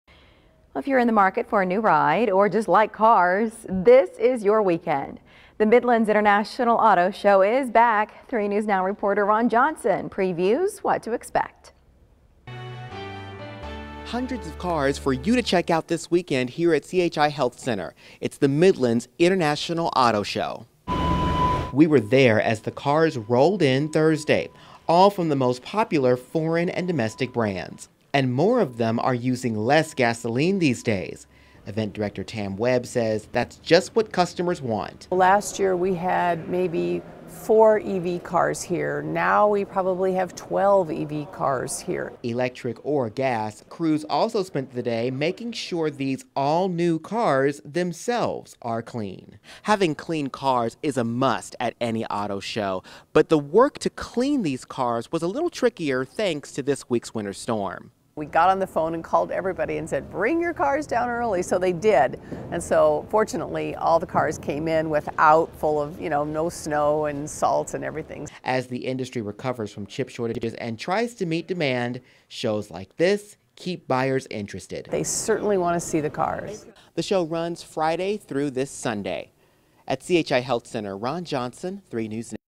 Midlands International Auto Show at CHI Health Center
Hundreds of cars will be on display this weekend at the CHI Health Center. Event director Tam Webb says spectators are becoming more interested in all electric/ hybrid vehicles.